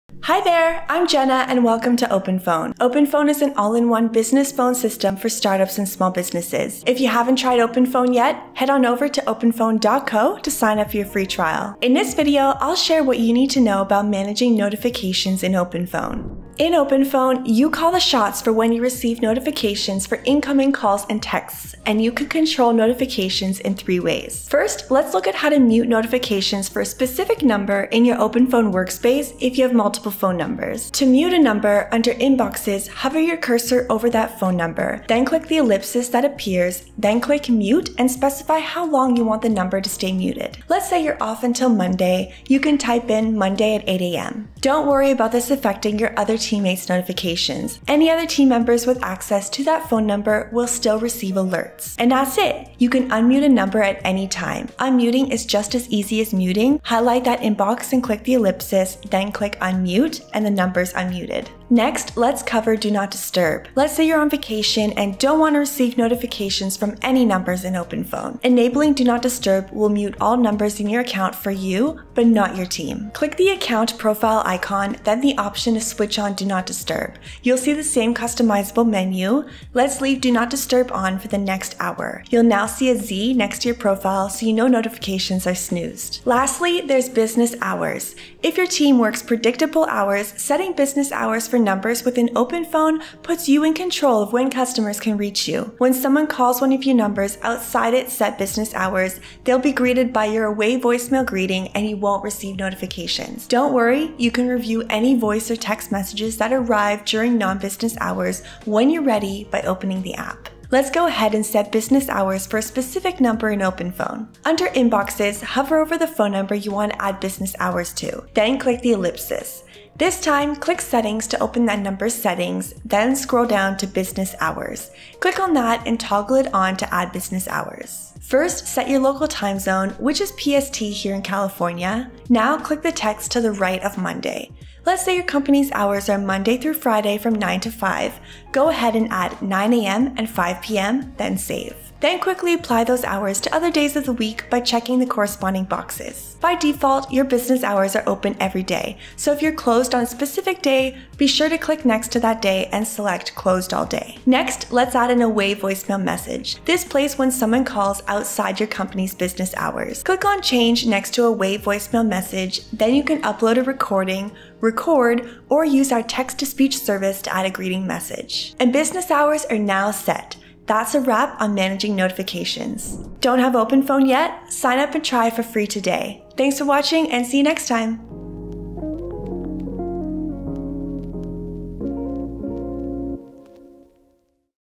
How to manage Quo (formerly OpenPhone) notifications
OpenPhone is the easiest way to get a business phone system that grows with you and your company.

In this video, we'll share ​​what you need to know about managing notifications in OpenPhone.

00:24 Muting call and text notifications
00:54 Unmuting a number
01:01 Turning on Do Not Disturb
01:28 Setting business hours

Don't have OpenPhone yet?